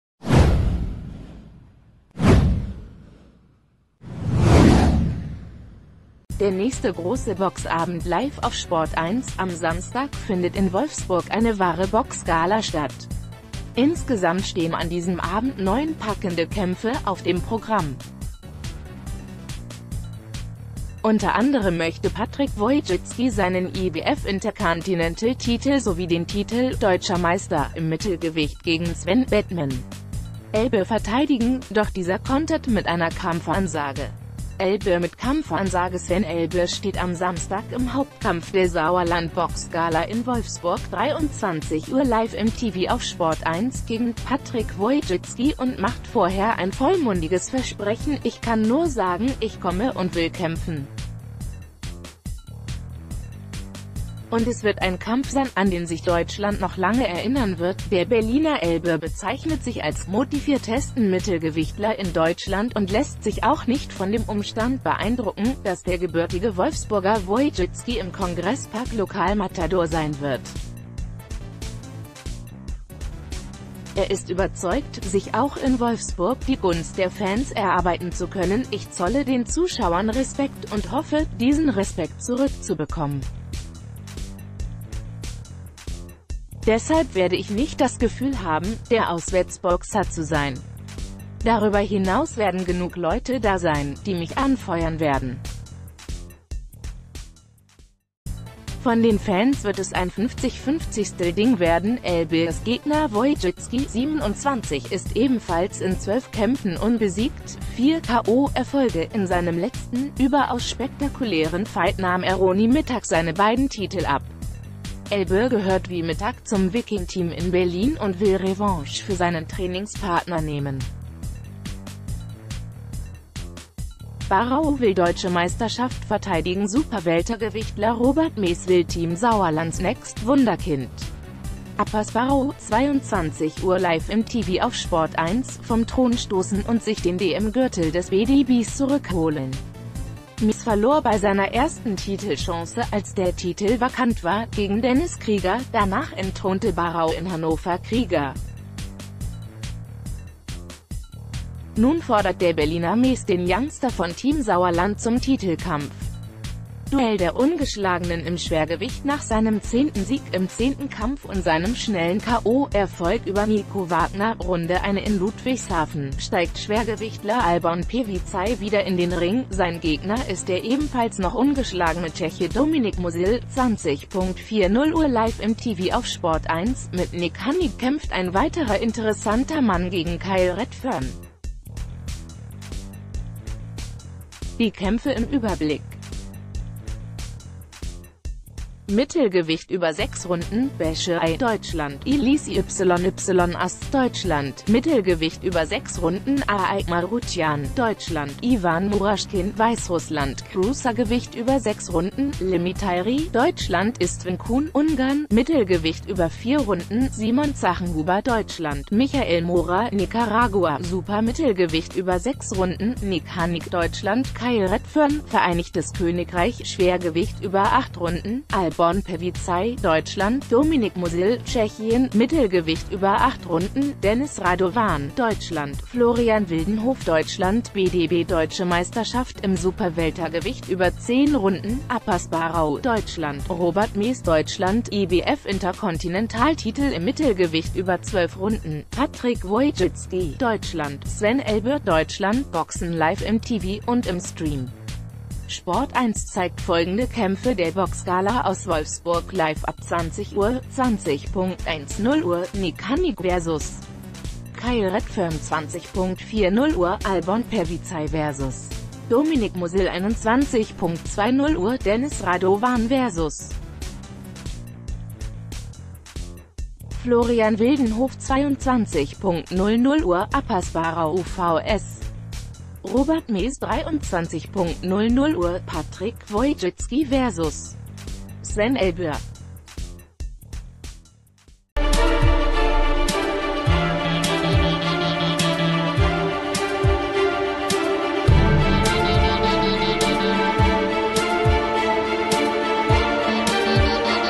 Boxen: Kämpfe aus Wolfsburg LIVE auf SPORT1 im TV und Livestream
Der nächste große Box-Abend LIVE auf SPORT1!Am Samstag findet in Wolfsburg eine wahre Box-Gala statt. Insgesamt stehen an diesem Abend neun packende Kämpfe auf dem Programm. Unter anderem möchte Patrick Wojcicki seinen IBF-Intercontinental-Titel sowie den Titel "Deutscher Meister" im Mittelgewicht gegen Sven "Batman" Elbir verteidigen - doch dieser kontert mit einer Kampfansage. Elbir mit KampfansageSven Elbir steht am Samstag im Hauptkampf der Sauerland-Boxgala in Wolfsburg (23 Uhr LIVE im TV a...